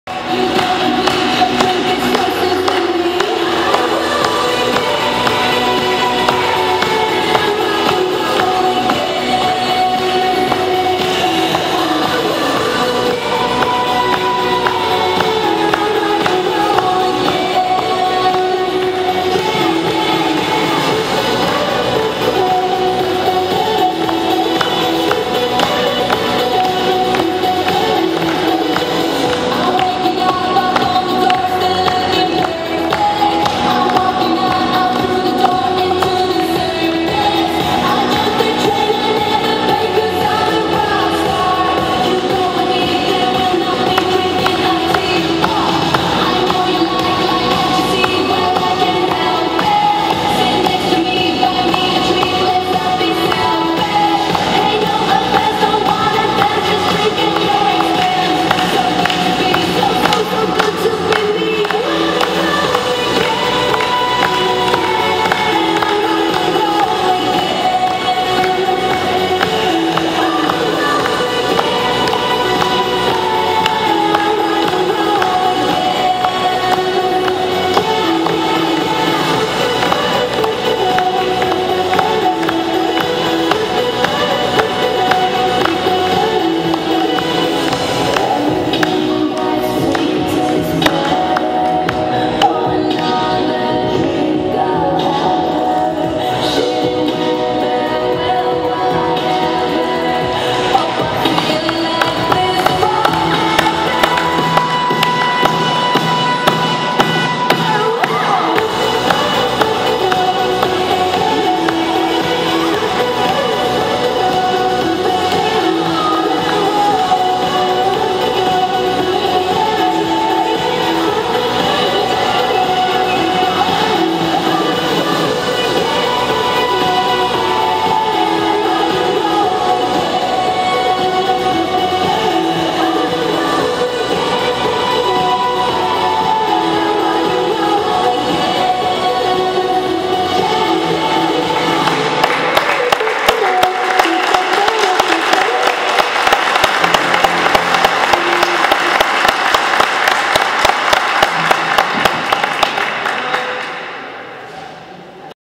El Campeón Argentino de freestyle en Villa Congreso(Parte 2)
Lezcano estuvo maravillándonos en el día del niño que celebro la Liga Barrial de Viedma en el gimnasio de Villa Congreso donde también hubo magos y juegos, sobre el final se repartieron golosinas a todos los niños.